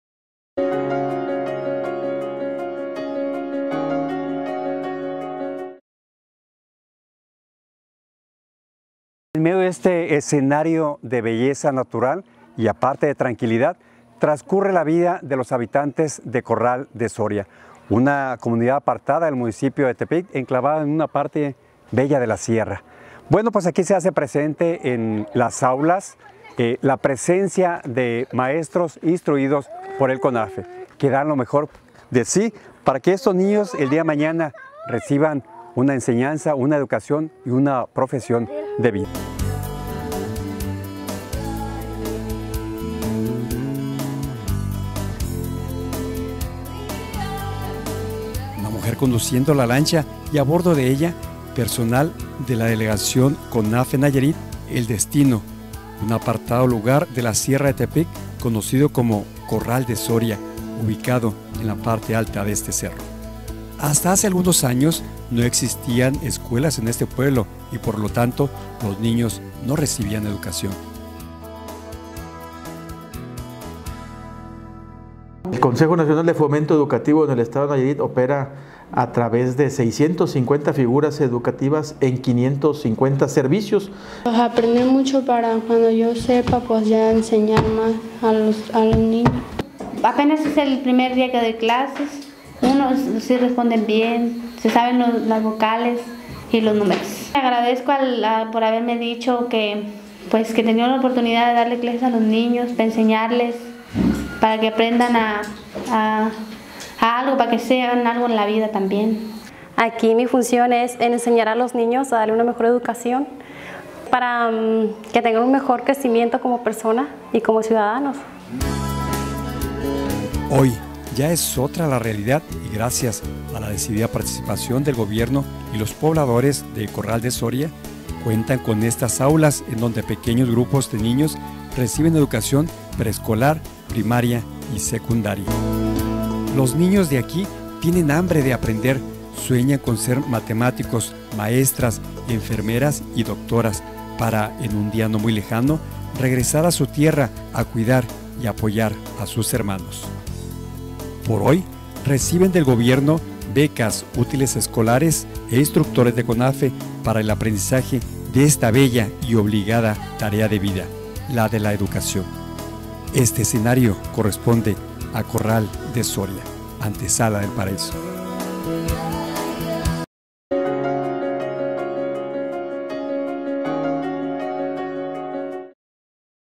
Corral de Soria, Nayarir una experiencia de la presencia del Conafe
El Consejo Nacional de Foment Educativo (Conafe) lleva a las comunidades más alejadas y marginadas del país educación y herramientas para hacer realidda el sueño de los niños.